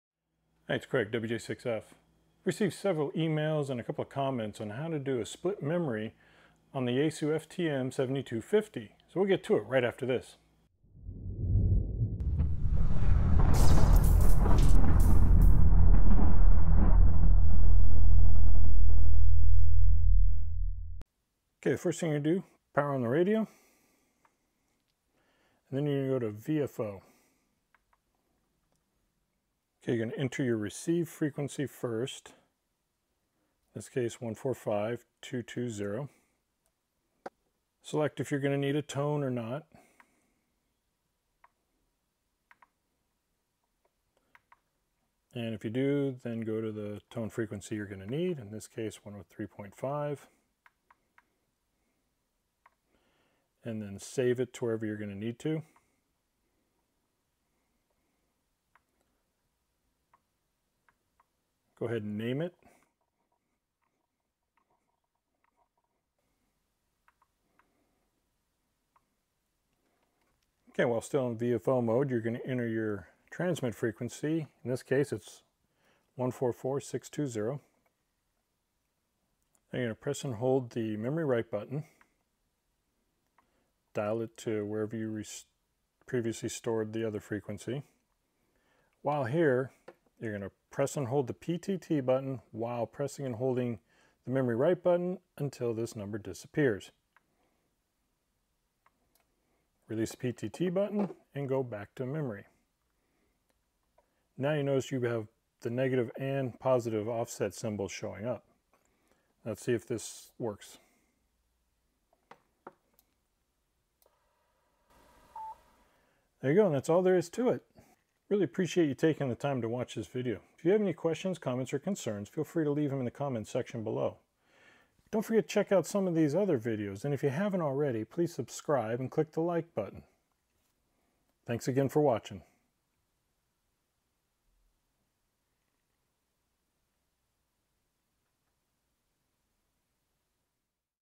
FTM-7250 Split Memory Programming
This video shows you how to program a split memory into your Yaesu FTM-7250.